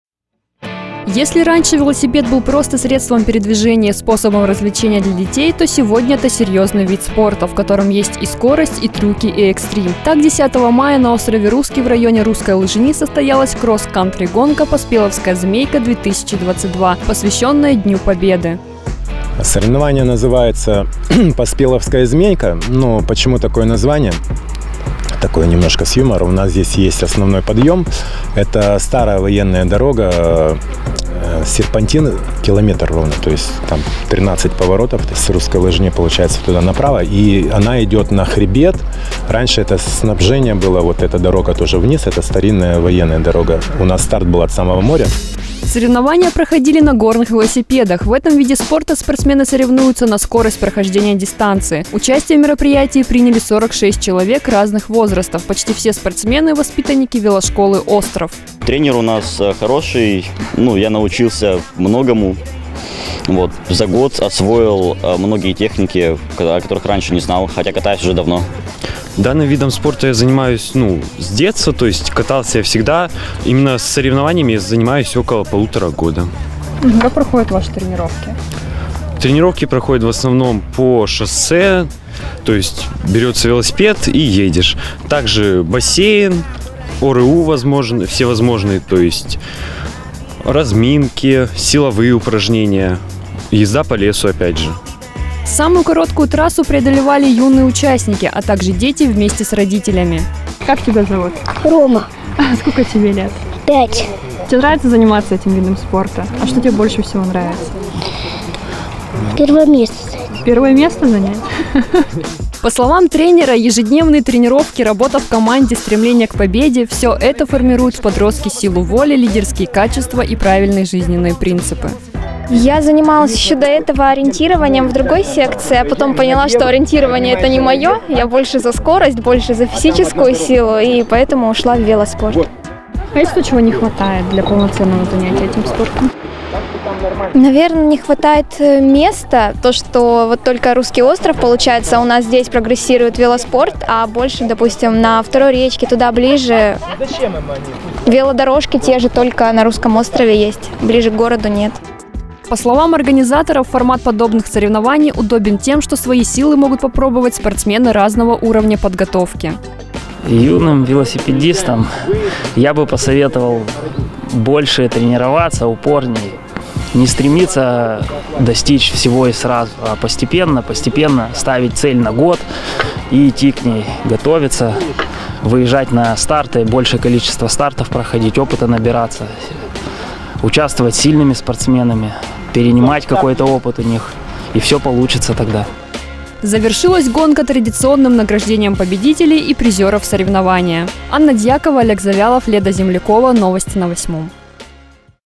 «Поспеловская змейка -2022»
Если раньше велосипед был просто средством передвижения и способом развлечения для детей, то сегодня это серьезный вид спорта, в котором есть и скорость, и трюки, и экстрим.